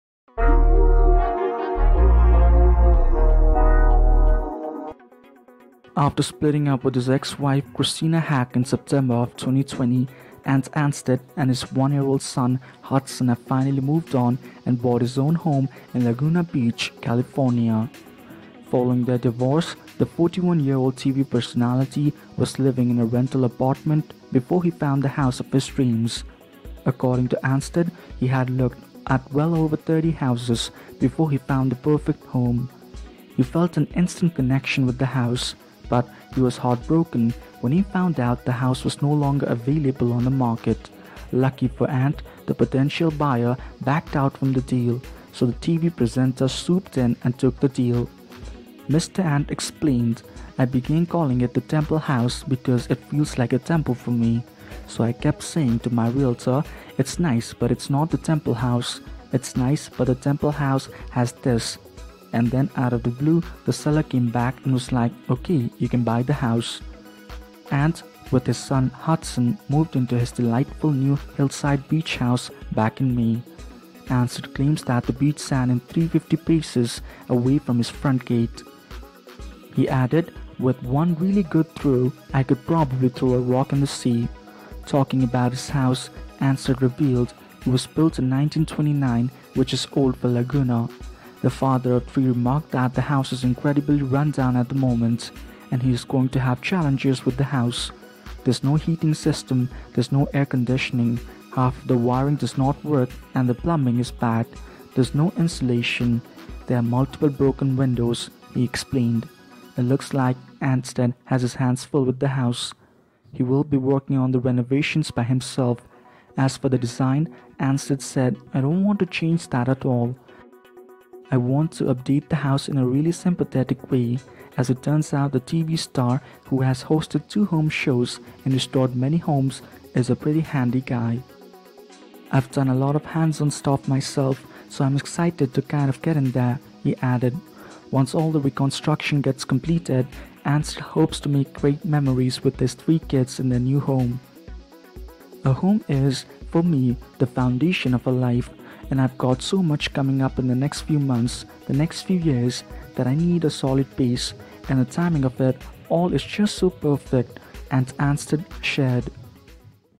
Ant Ansted Moves To His Dream House In Laguna With Son Hudson Anstead
After splitting up with his ex-wife Christina Haack in September, Ant Anstead and his 1-year-old son, Hudson, have finally moved on and bought his own home in Laguna Beach, California.
According to Anstead, he had "looked at well over 30 houses" before he found the perfect home.
But, he was "heartbroken" when he found out the house was no longer available on the market.
Ant, with his son Hudson, moved into his delightful new hillside beach house back in May.
Once all the reconstruction gets completed, Anstead hopes to make great memories with his three kids in their new home.